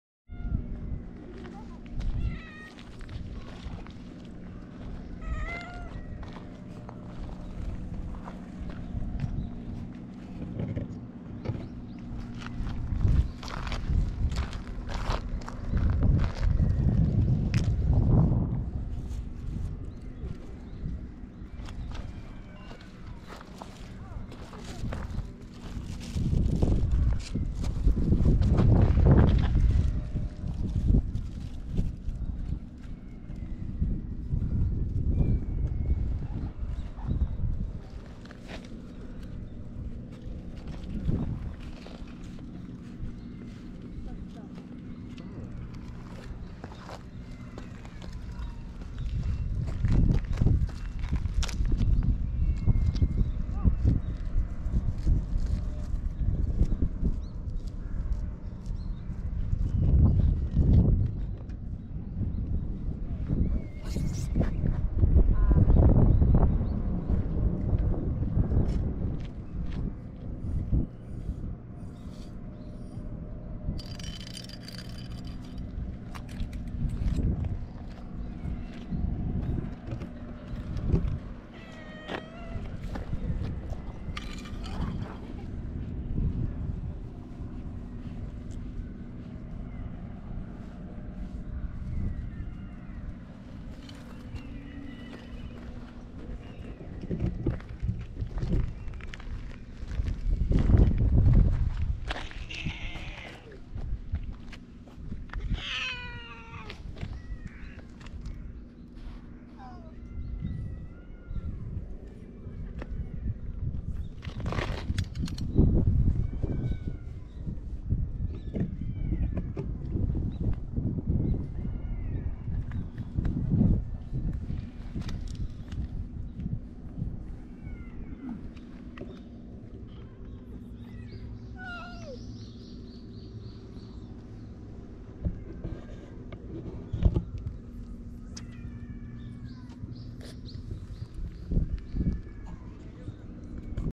防波堤で釣りをする人の近くには人懐っこい野良猫と出会えてうれしい
関連動画

公園の蛇口から水を飲む黒い野良猫「スミマセン、水飲みたいので蛇口開けて下さい。」私「あ。はい・・・」

GH010080